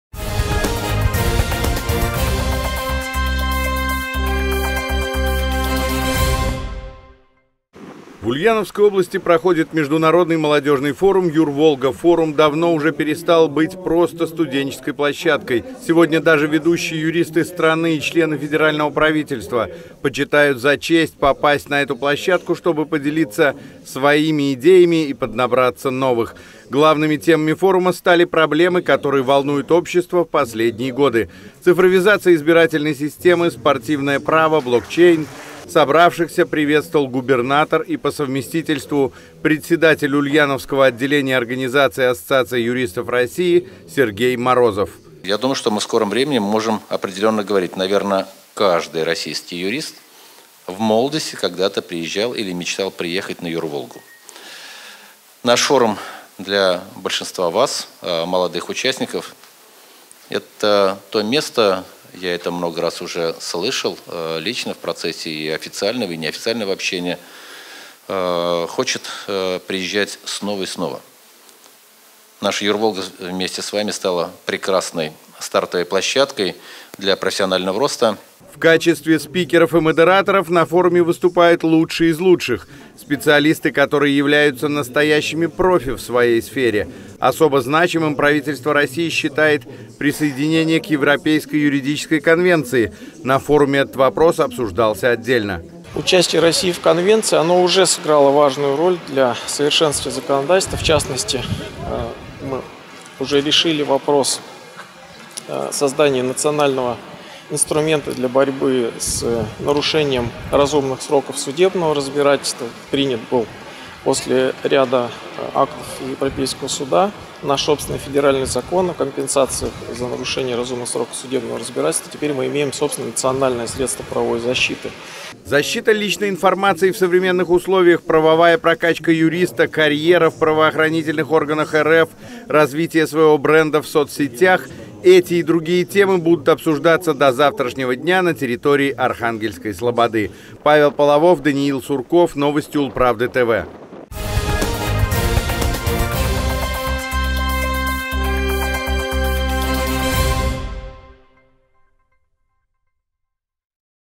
Лучшая площадка для молодых юристов открылась в Ульяновской области
В регионе проходит международный молодежный форум «ЮрВолга».